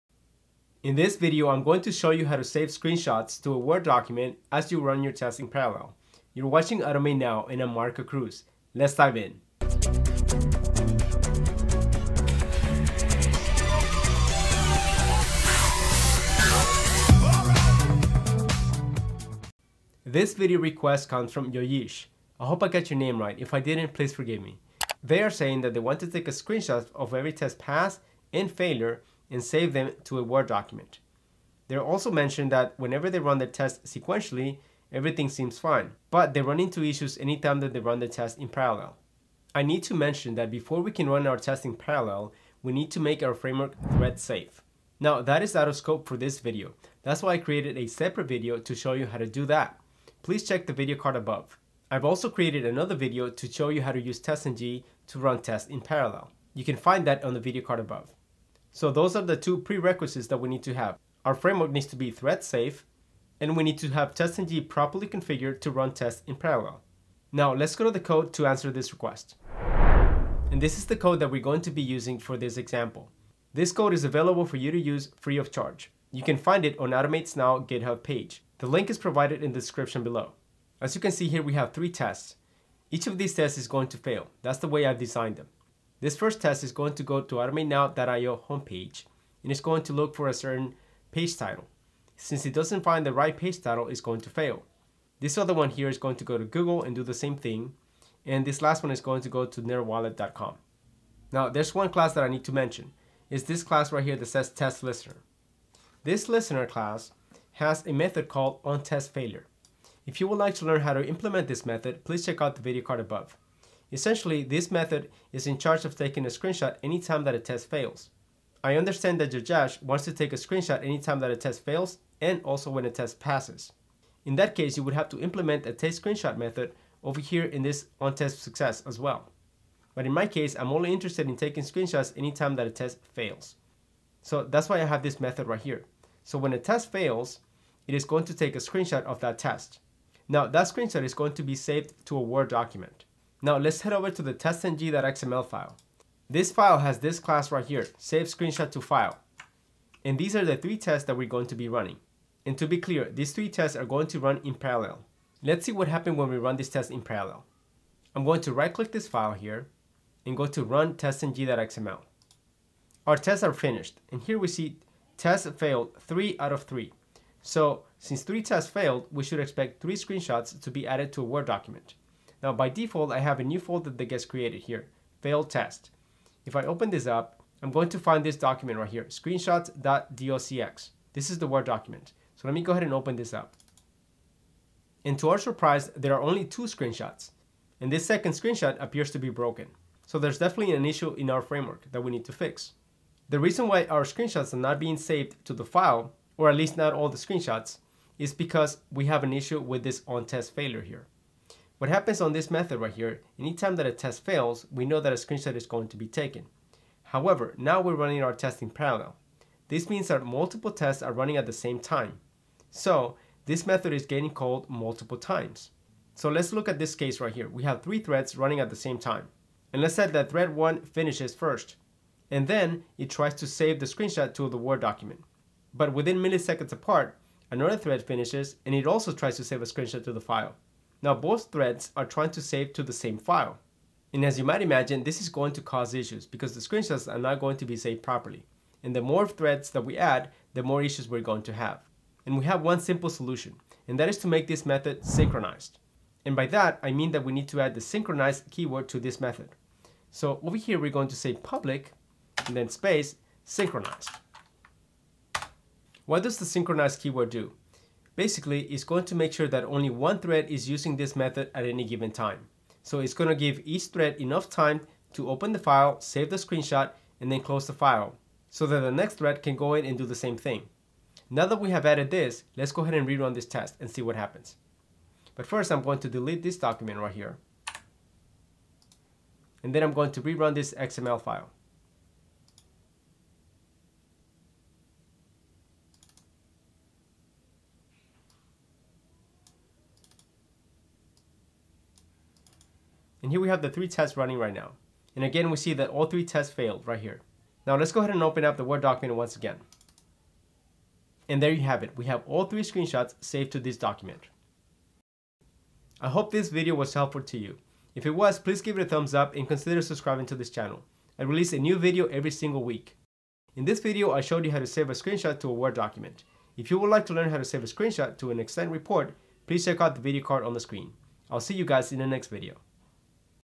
Selenium parallel execution | Take screenshot | automateNow
Learn how to run Selenium parallel execution, take a screenshot of failed tests and save them to a Word document.

Taking screenshots of failed tests and saving them to a file is a breeze so long as you are running your tests sequentially. However, this can become challenging when you run your tests in parallel.

In such a case, you must take the proper steps to make your Selenium tests thread-safe.

This video tutorial will teach you how to use Java's synchronized keyword to support the taking of a screenshot during parallel execution.

👨‍💻Learn Selenium like a pro with our highly-rated courses!

📑 CHAPTERS
0:00 ~ Intro
0:39 ~ Prerequisites For Parallel Execution
1:10 ~ Test Scenario
1:41 ~ onTestFailure() Method
2:23 ~ testng.xml File
3:13 ~ Synchronized Methods
5:05 ~ Outro